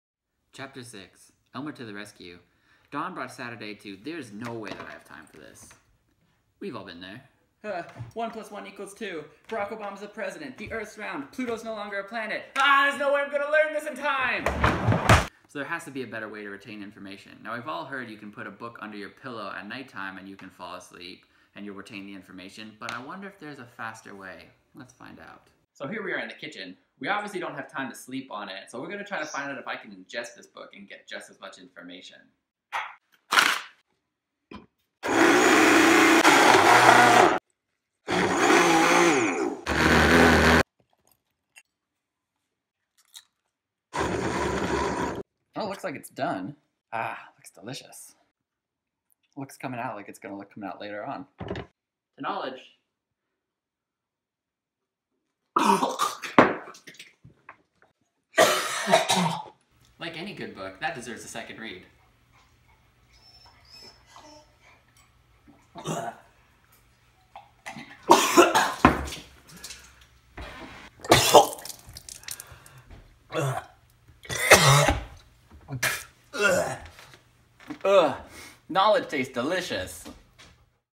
Break Time: Book Reading
Coltan tries to cheat the system and find a faster way to get the information out of a book.